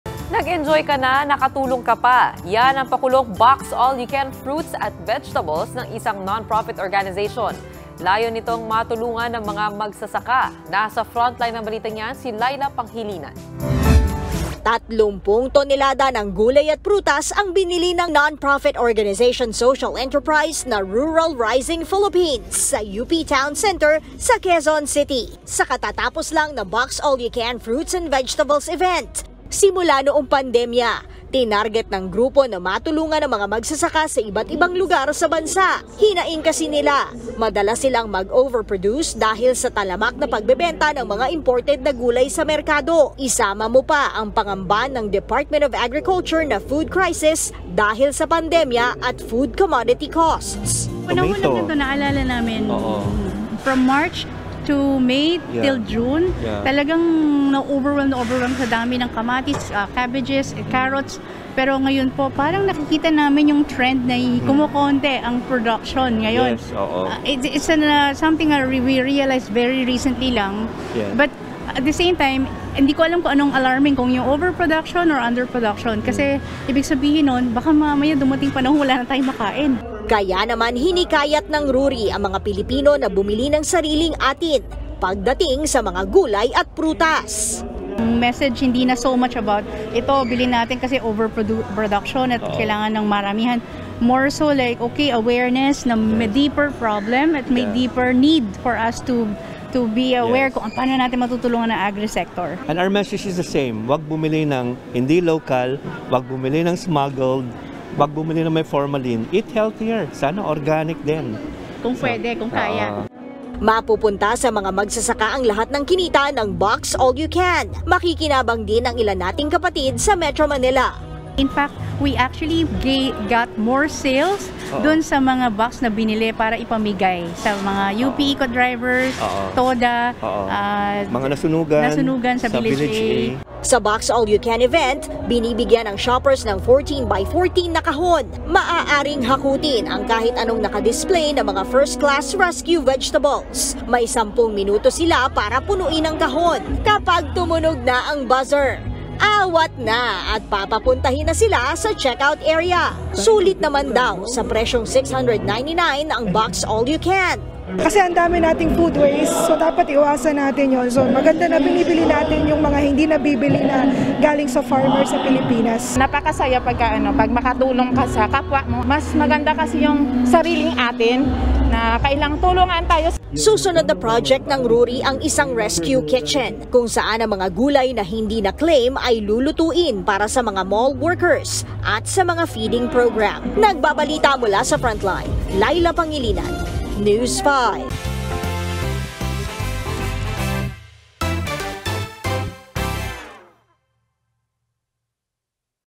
Box-all-you-can fruits and vegetables, pakulo ng isang NGO
FrontlineSaUmaga | Nag-enjoy ka na, nakatulong ka pa. ‘Yan ang pakulong box-all-you-can fruits and vegetables ng isang non-profit organization. Layon nitong matulungan ang mga magsasaka. | via Laila Pangilinan